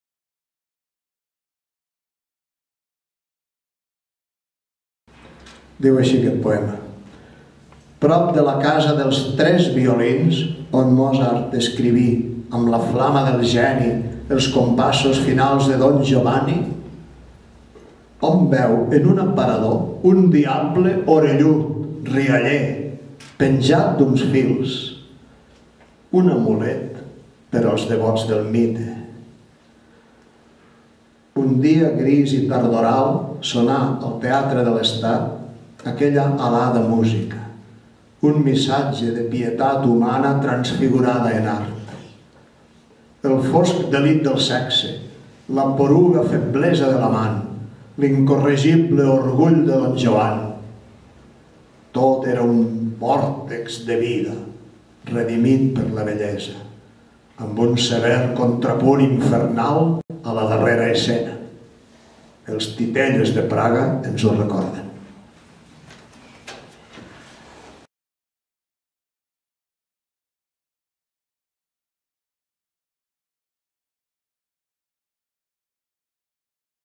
Jordi Pàmias - Don Giovanni
Jordi Pàmias recita el poema "Don Giovanni" a la Biblioteca Guinardó-Mercè Rodoreda durant l'acte del dia 17 d'octubre de 2013 dins la 21a edició del cicle "De Pensament, Paraula i Obra", organitzat per l'Aula de Poesia de Barcelona i el Districte d'Horta-Guinardó de Barcelona. L'acompanya el vicepresident de l'Aula de Poesia, Eduard Sanahuja.